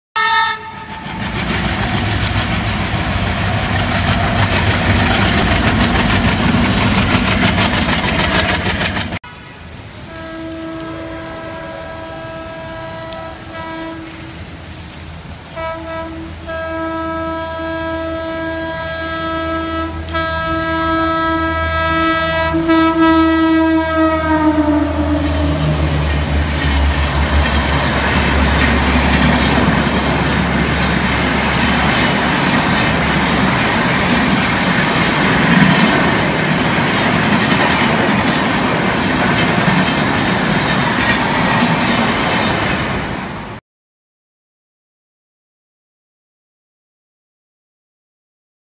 Rainy Evening Show with Shatabdi Express - Indian Railways.
Train no 12010 Ahmedabad - Mumbai Central Shatabdi Express with Vadodara WAP7i passing Sachin At MPS.

Location - Sachin,Gujarat,India(Western Railways),

Happy and Safe Railfaning,